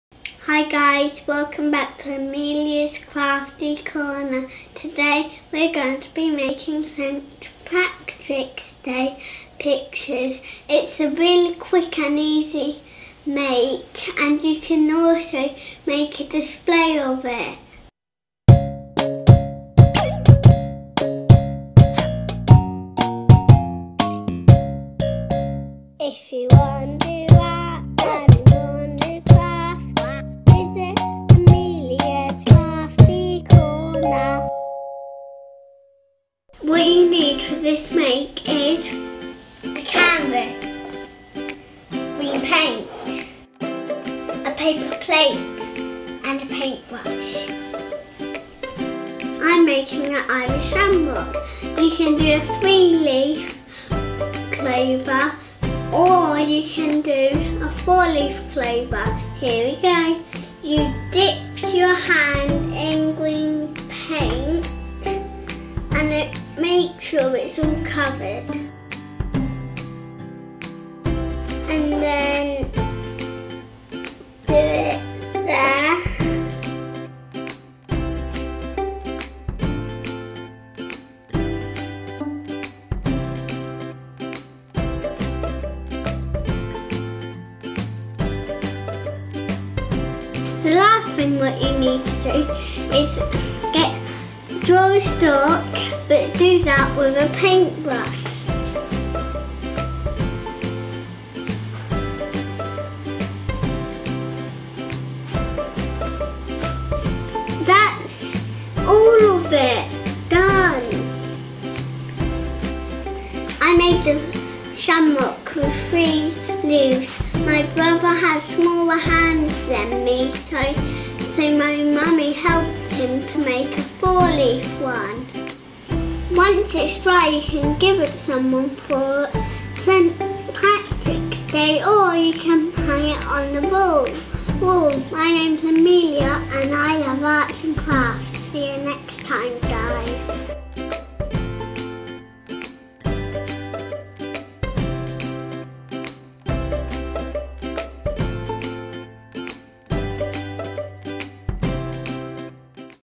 Shamrock St Patrick's Day Picture - Amelia's Crafty Corner
Watch me make a really quick and simple shamrock picture to celebrate St Patrick's Day. All you need is a canvas, some green paint, a paper plate and a paintbrush.